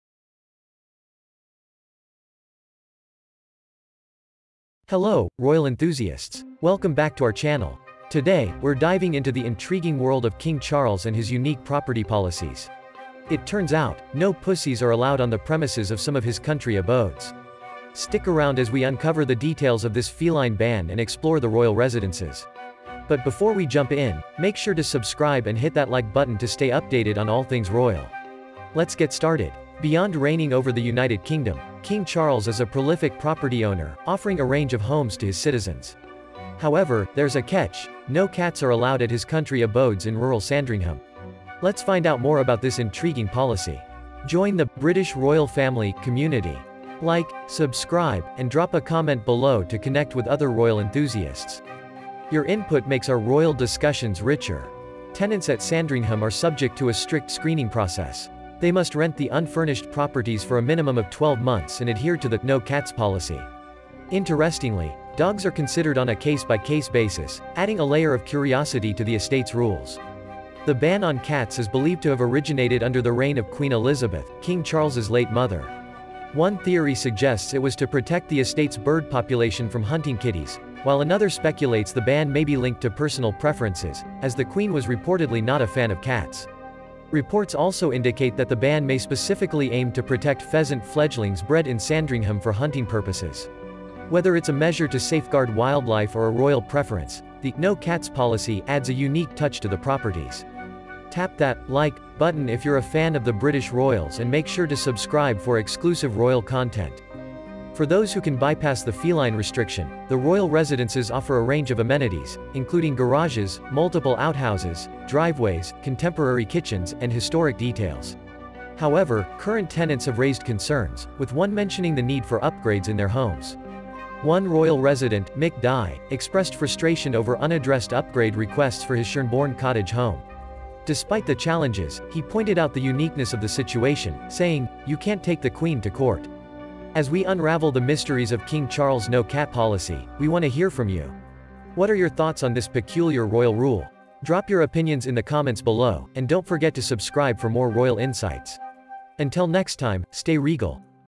ROYAL SHOCKER! KING CHARLES STRICTLY FORBIDS HAVING CATS IN HIS RENTAL HOMES
Hello, royal enthusiasts! Today, we're diving into the intriguing world of King Charles and his unique property policies. It turns out, no pussies are allowed on the premises of some of his country abodes. Stick around as we uncover the details of this feline ban and explore the royal residences. Let's get started!

Beyond reigning over the United Kingdom, King Charles is a prolific property owner, offering a range of homes to his citizens. However, there's a catch—no cats are allowed at his country abodes in rural Sandringham. Let's find out more about this intriguing policy.

Tenants at Sandringham are subject to a strict screening process. They must rent the unfurnished properties for a minimum of 12 months and adhere to the 'no cats policy.' Interestingly, dogs are considered on a case-by-case basis, adding a layer of curiosity to the estate's rules.

The ban on cats is believed to have originated under the reign of Queen Elizabeth, King Charles' late mother. One theory suggests it was to protect the estate's bird population from hunting kitties, while another speculates the ban may be linked to personal preferences, as the Queen was reportedly not a fan of cats.

Reports also indicate that the ban may specifically aim to protect pheasant fledglings bred in Sandringham for hunting purposes. Whether it's a measure to safeguard wildlife or a royal preference, the 'no cats policy' adds a unique touch to the properties.

For those who can bypass the feline restriction, the royal residences offer a range of amenities, including garages, multiple outhouses, driveways, contemporary kitchens, and historic details. However, current tenants have raised concerns, with one mentioning the need for upgrades in their homes.

One royal resident, Mick Dye, expressed frustration over unaddressed upgrade requests for his Shernborne cottage home. Despite the challenges, he pointed out the uniqueness of the situation, saying, 'You can't take the Queen to court.'

As we unravel the mysteries of King Charles no-cat policy, we want to hear from you. What are your thoughts on this peculiar royal rule? Until next time, stay regal!